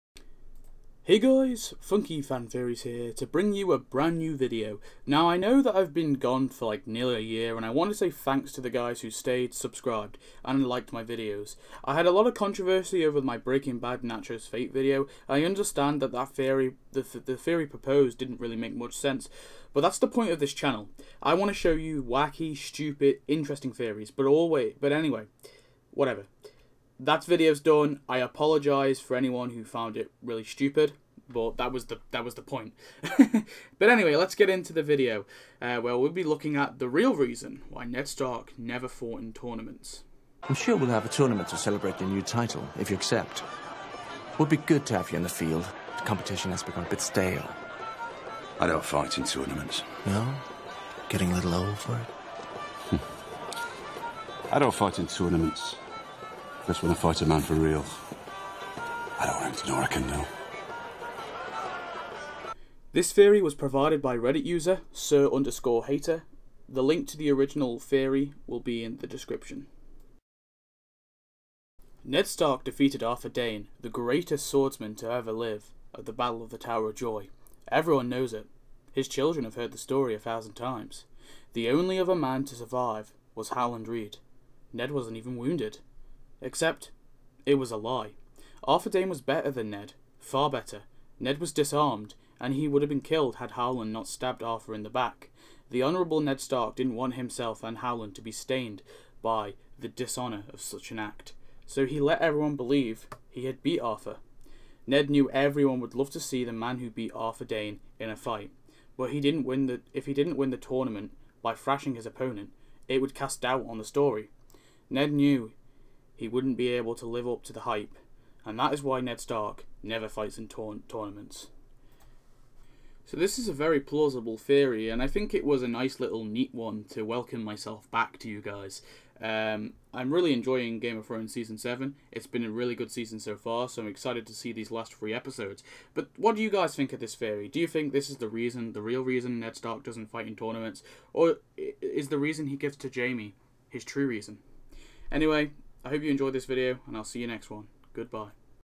Game Of Thrones Fan Theory | Why Ned Stark Never Fought In Tournaments.
“I don't fight in tournaments because when I fight a man for real, I don't want him to know what I can do.”

Ned Stark defeated Arthur Dayne, the greatest swordsman to ever live, at the battle of the Tower of Joy. Everyone knows it. His children have heard the story a 1,000 times. The only other man to survive was Howland Reed, Ned wasn't even wounded.

Except it was a lie.

Arthur Dayne was better then Ned, FAR better. Ned was disarmed and would have been killed had Howland not stabbed Arthur in the back. The honorable Ned Stark didn't want himself and Howland to be stained by the dishonor of such an act. So he let everyone believe he had beat Arthur.

Ned knew everyone would love to see the man who beat Dayne in a fight. But if he didn't win the tournament handedly it would cast doubt on the story. Ned new he wouldn't be able to live up to the hype. That is why Ned Stark never fights in tourneys.

Check Out the Theory